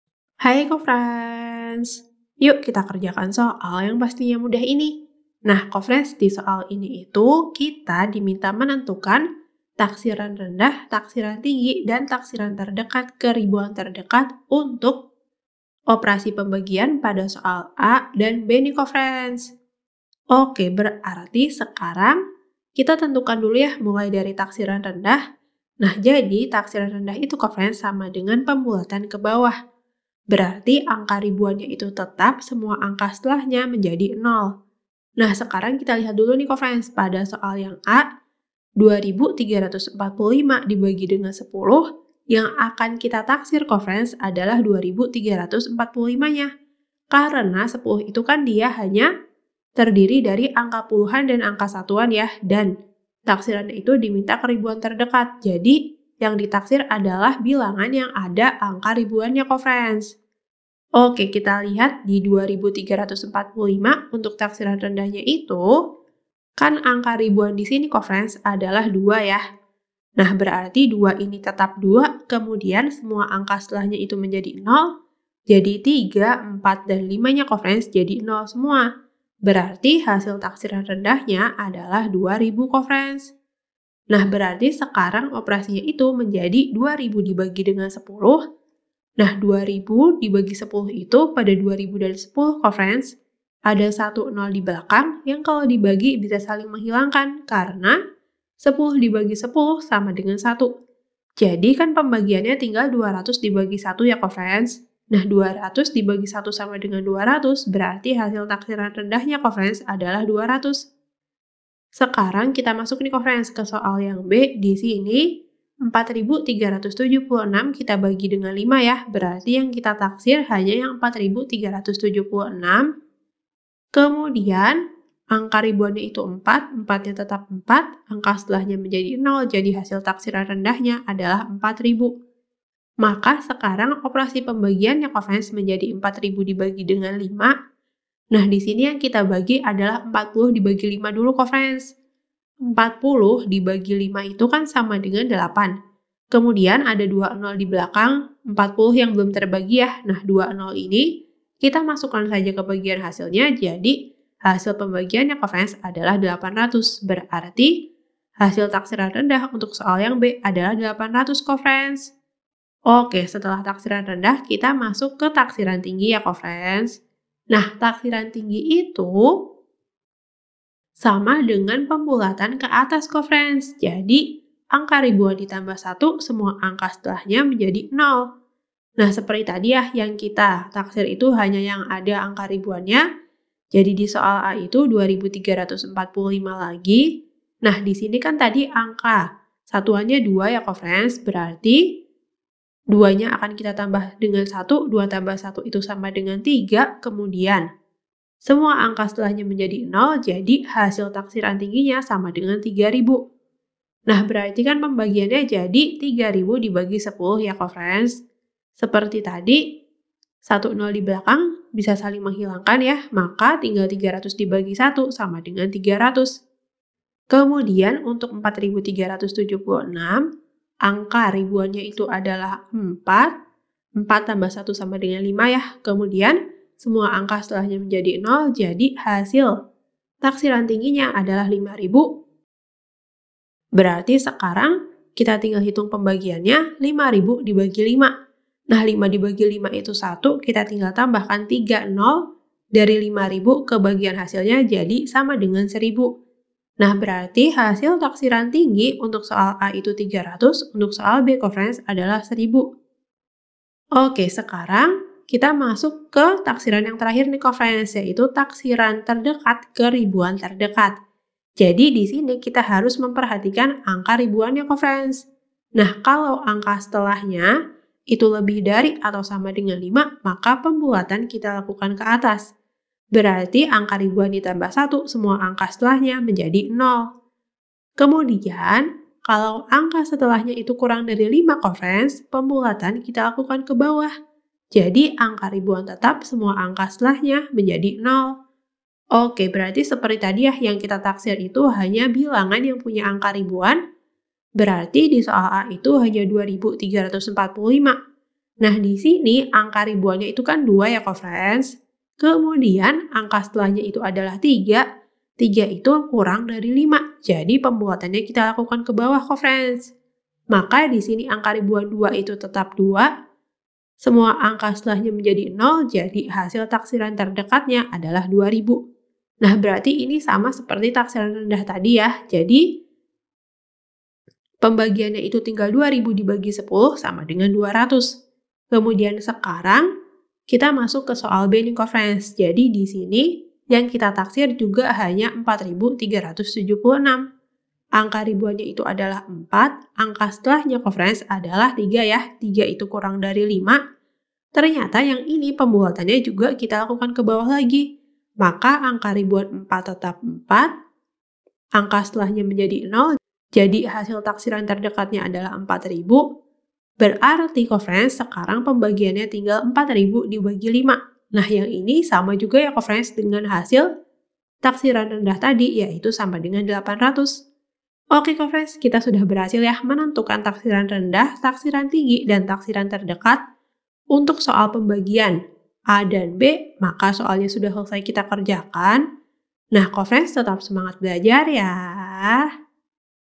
Tentukan taksiran rendah, taksiran tinggi, dan taksiran terdekat ke ribuan terdekat! a. 2.345: 1...
Tentukan taksiran rendah, taksiran tinggi, dan taksiran terdekat ke ribuan terdekat!

Topik atau Materi: Pembulatan ke Ribuan(atau lebih) Terdekat - Aproksimasi / Pembulatan - Aritmatika - Matematika Kelas 4 SD - K-13 Revisi

Sumber Soal: Modul Matematika Gemilang Kelas 4A Smt.1, K-13 Revisi, MSH, 2019 , N/A

00:00 : Bedah Soal
01:00 : Konsep, rumus dan pengertian Pembulatan ke Ribuan(atau lebih) Terdekat
01:18 : Langkah penyelesaian soal
05:23 : Jawaban akhir
05:53 : Penutup